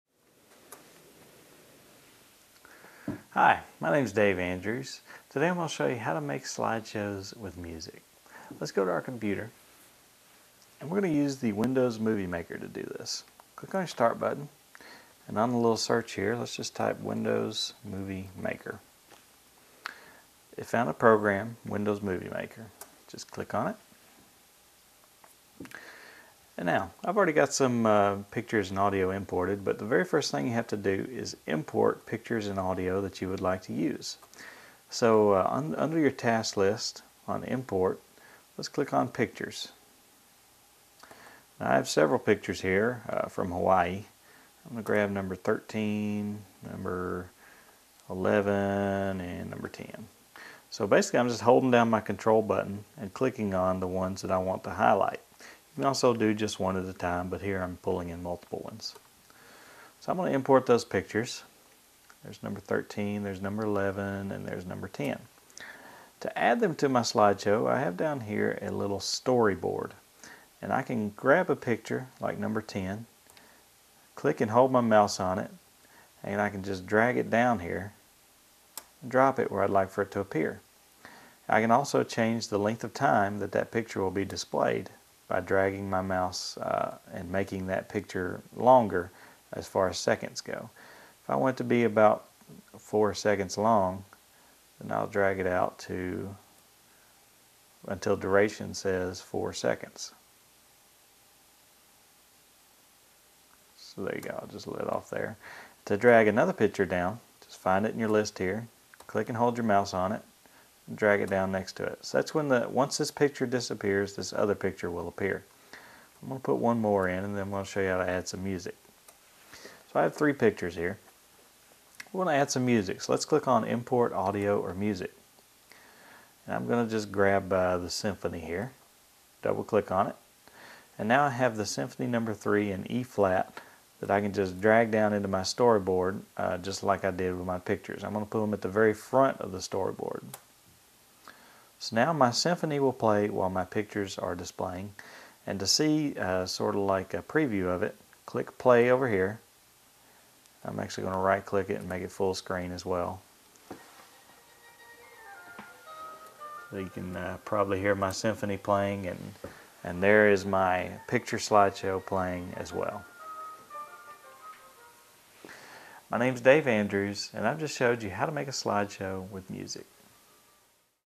Computers Tips & Advice : How to Make Slideshows With Music
To make slideshows with music on a computer, open Windows Movie Maker, import pictures and audio, drag and drop pictures into a slideshow, and drag imported music into the slideshow. Preview a slideshow that includes pictures and music with instructions from a software developer in this free video on computer programs.